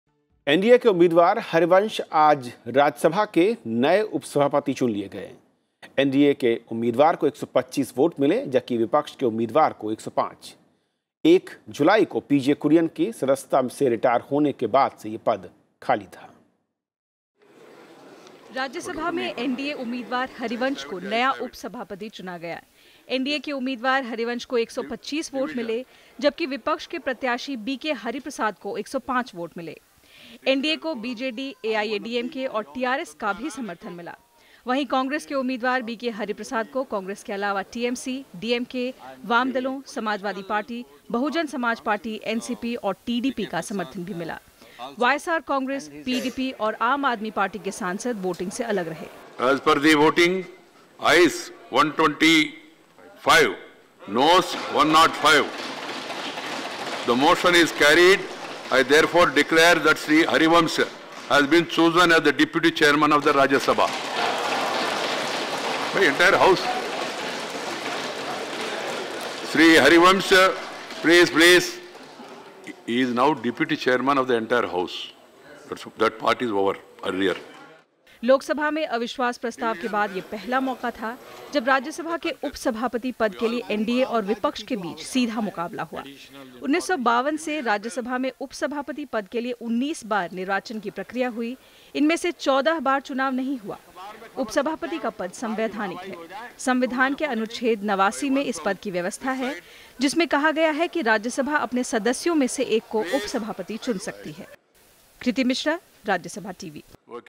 First time MP, Harivansh Narayan Singh is Rajya Sabha's new Deputy Chairman
NDA’s Harivansh Narayan Singh was elected as Rajya Sabha Deputy Chairman on August 9, 2018. He won the election with 125 votes against opposition’s BK Hariprasad who secured 105 votes.